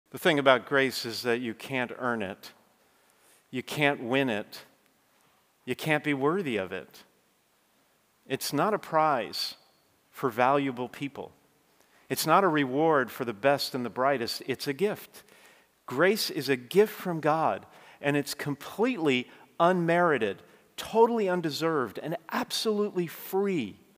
Grace is a gift and it's absolutely free
This is a series about God's grace. What grace is, what it does for us, and how we can get it, keep it, and pass it along to others. Grace prepares us for eternal life with God and helps to live better now. Join us for a series where you don't have to prove your worth, but simply live graciously!